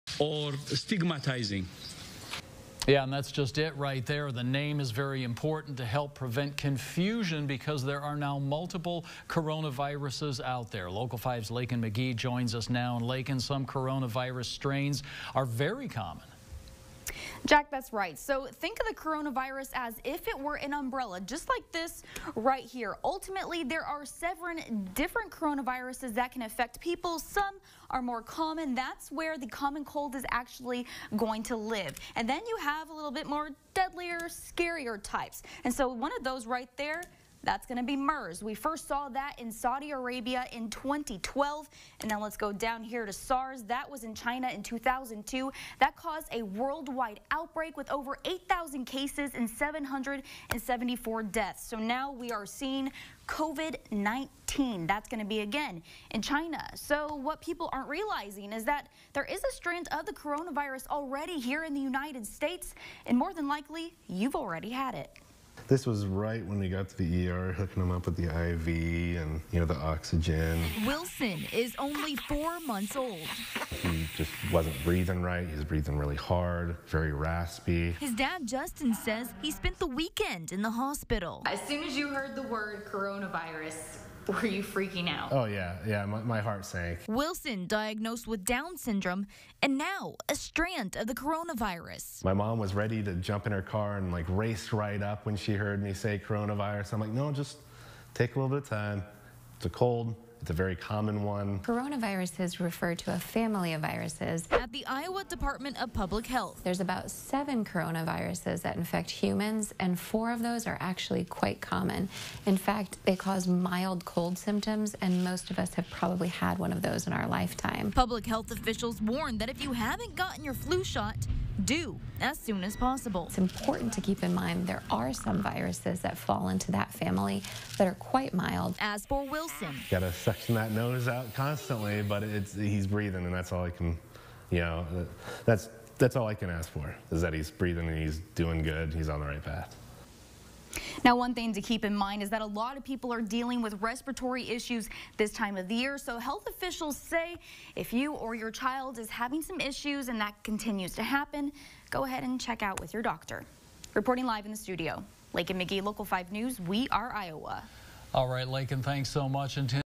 !EXPOSED! You've had the CORONAVIRUS at some point in your life!
{YOU'VE HAD THE CORONAVIRUS SOME POINT IN YOUR LIFE} - There are 7 different types of coronavirus and chances are you've had it!